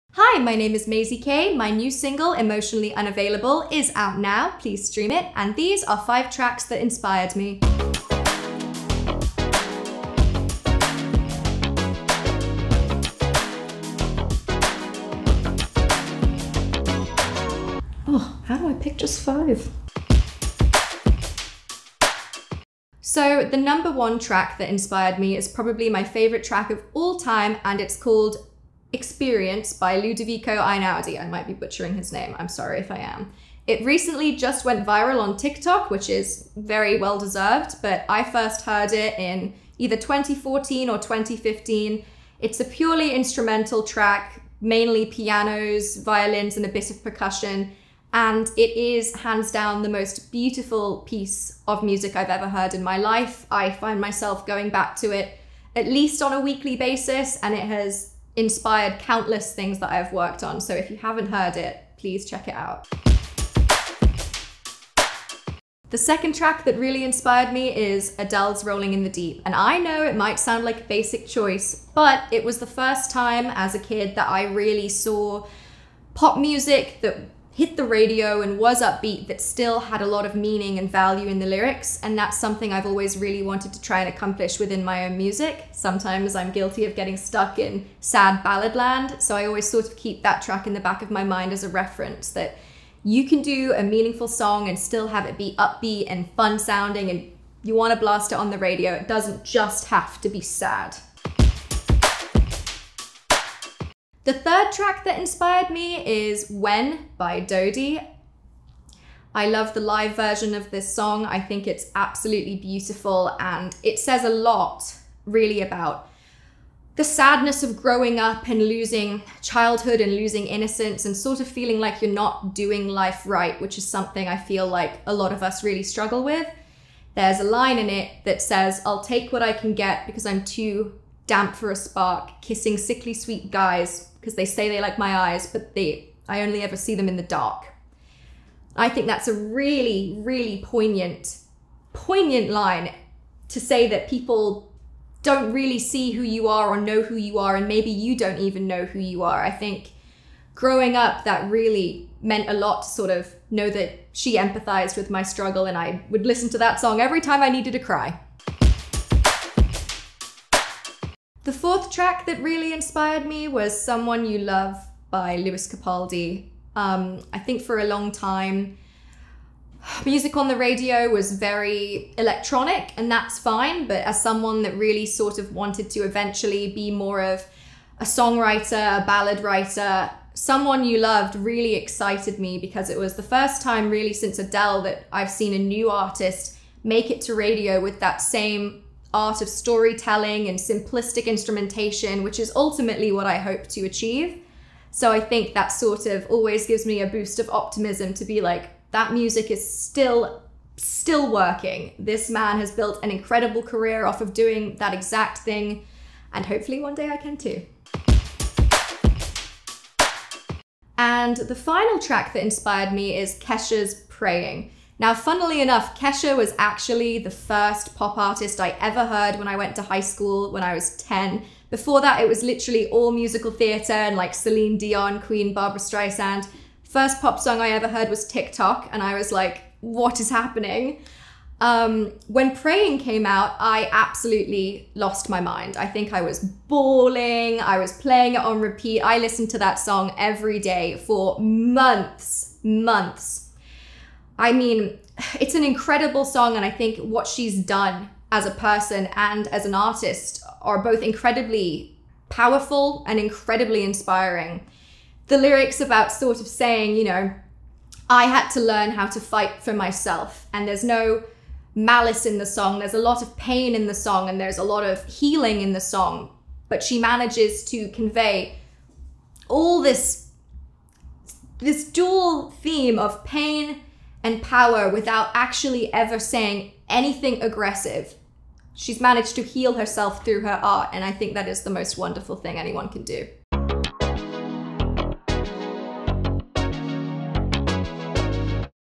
5 Tracks That Inspired Maisy Kay
In the first episode of "5 Tracks That Inspired Me," Maisy Kay breaks down some of her favorites, including songs by Adele, Dodie, and Lewis Capaldi.